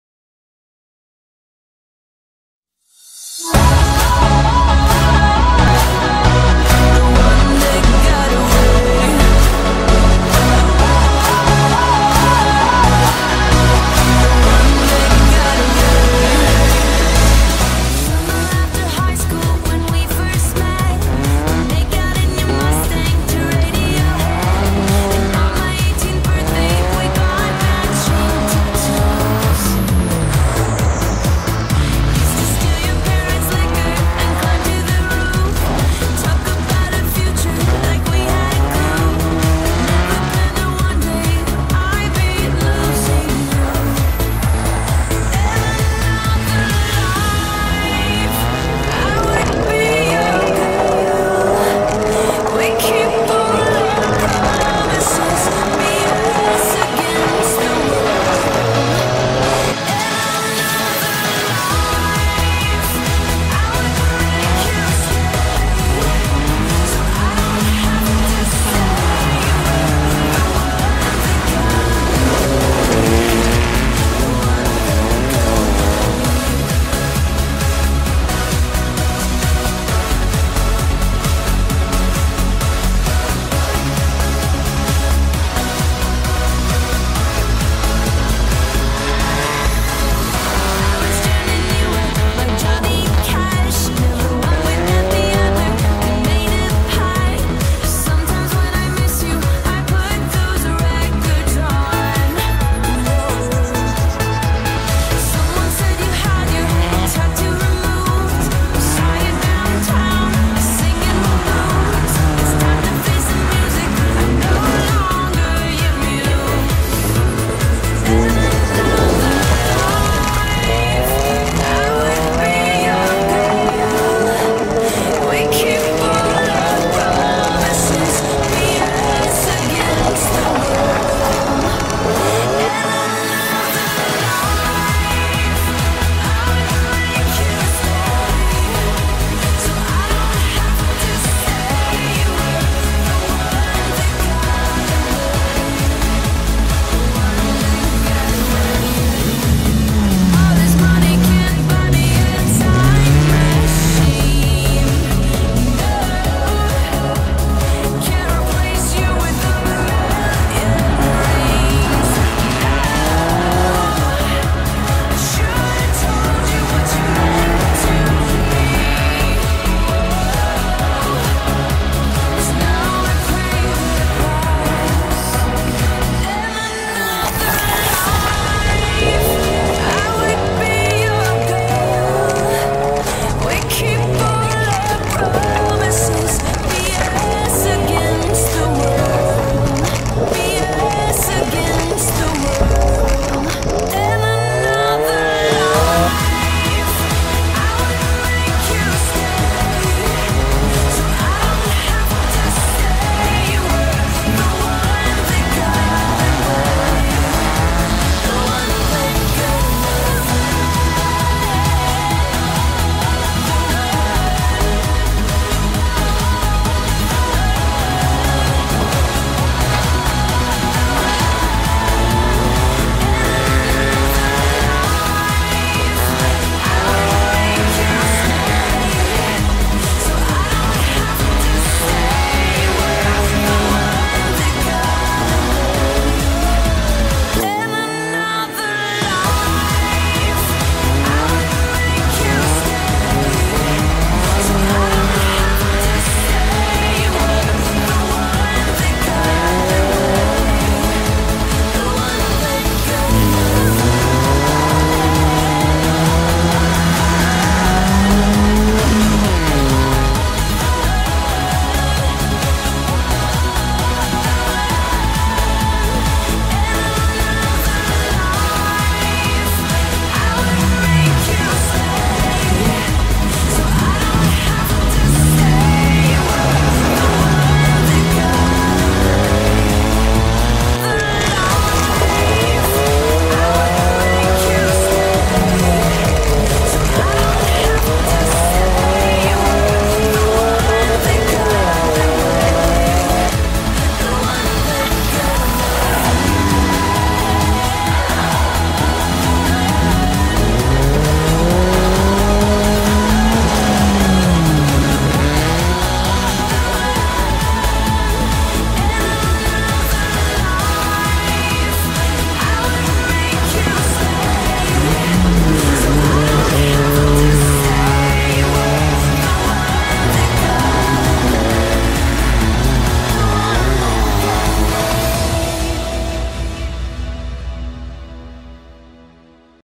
Salas Rally Rent | Tramo Tierra de Santillana del Mar 2022 | Ford Focus
Resumen de la participación del equipo Salas Rally Rent formado pro  Álvaro Salas y Rafael Gil en el Tramo Tierra de Santillana del Mar 2022.